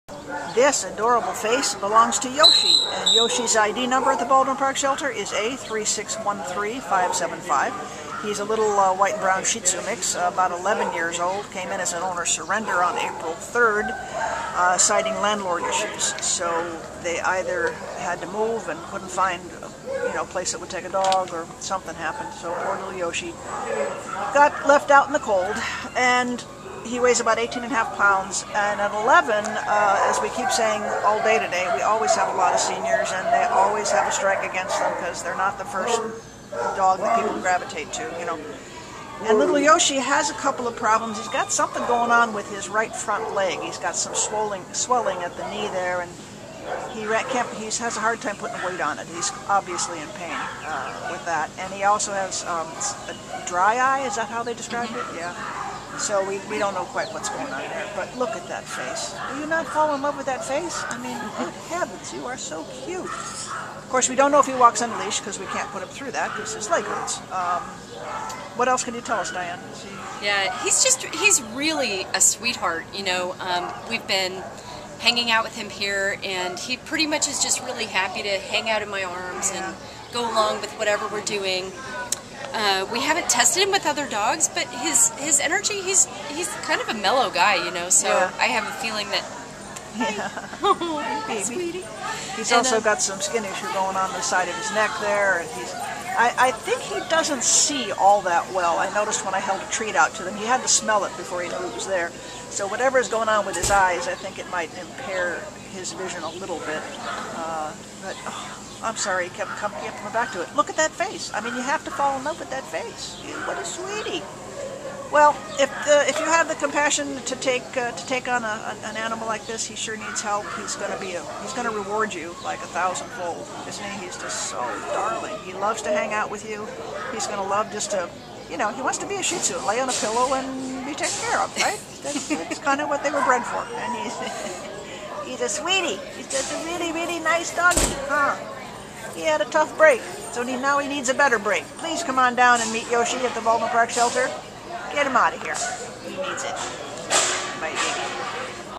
A3613575 Yoshi | Shih Tzu
A3613575 Yoshi is an amiable 11-year-old male white-and-brown Shih Tzu who came to the Baldwin Park Animal Care Center on April 3rd because of a landlord dispute. Weighing 19 lbs, Yoshi is an adorable little guy who shows signs of housebreaking, takes treats gently, and loves everyone he meets. He is a lapdog supreme! Yoshi’s coat has been neglected but should be wonderful when it grows out. He should see an outside vet for a dental exam and also have his right front leg looked at, since it seems to be painful and he’s reluctant to put it down. This is a wonderful, loving, outgoing dog with lots of love left to give. He’ll make an ideal pet for anyone who has love to give back.